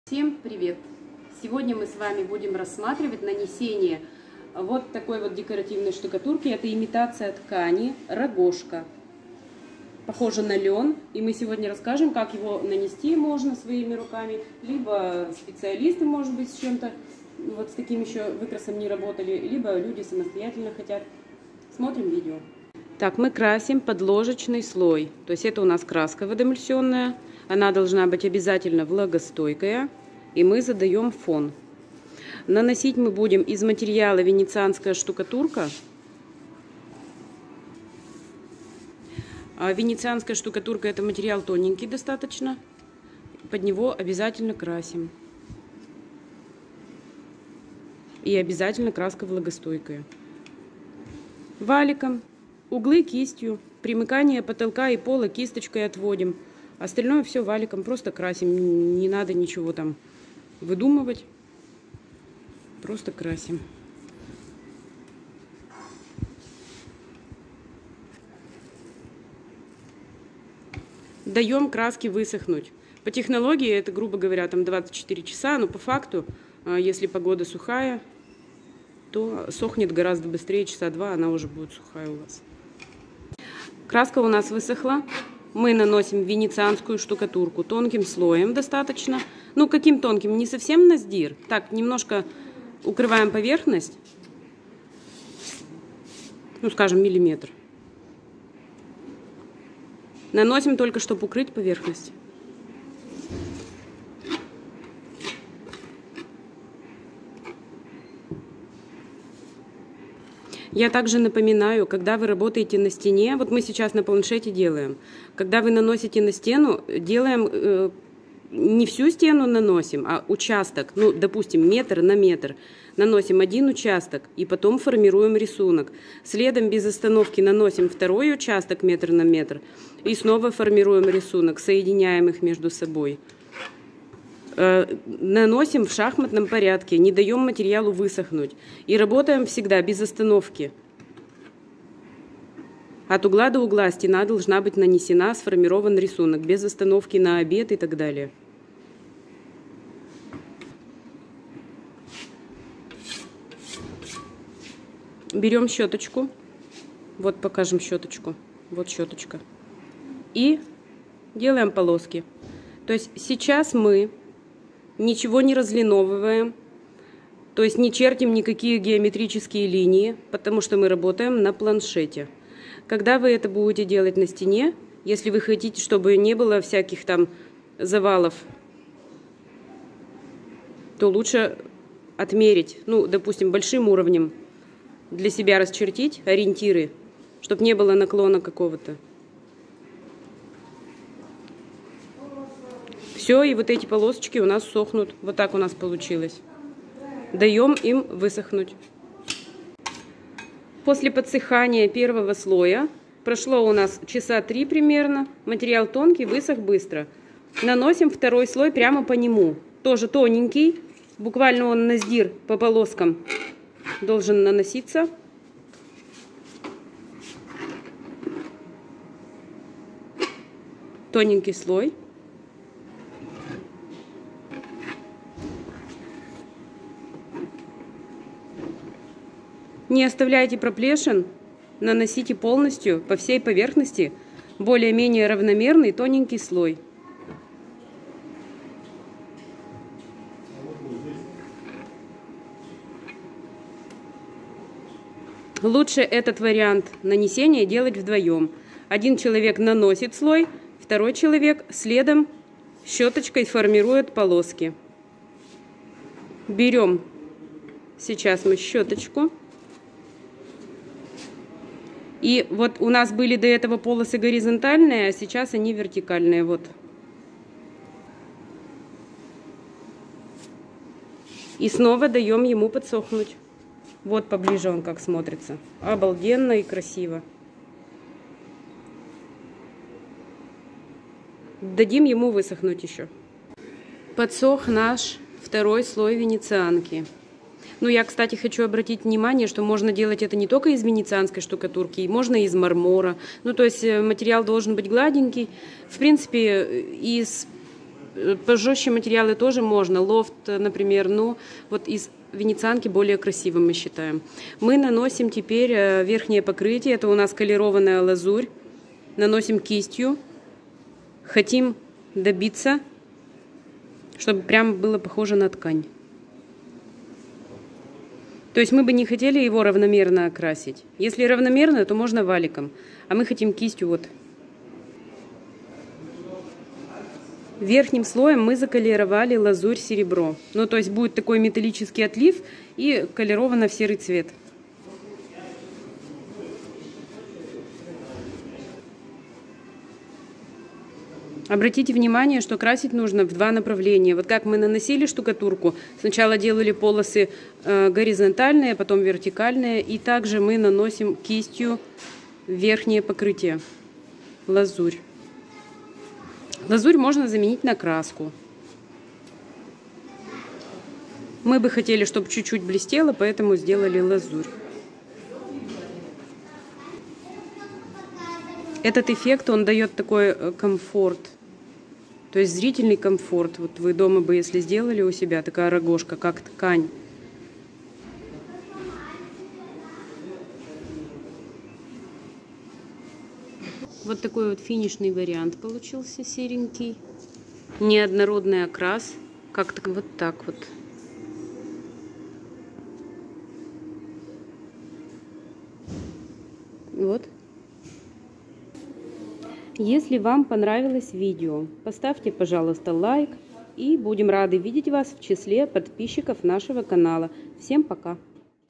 Декоративная штукатурка/Имитация ткани/Рогожка
Очень приятный и спокойный вариант нанесения. К тому же он действительно легкий. Нужен шпатель, щетка и кисть. Использовать можно декоративную штукатурку пастообразную любую. Без крупных гранул, фракций и т.п. Можно колеровать в массе, можно окрасить поверх.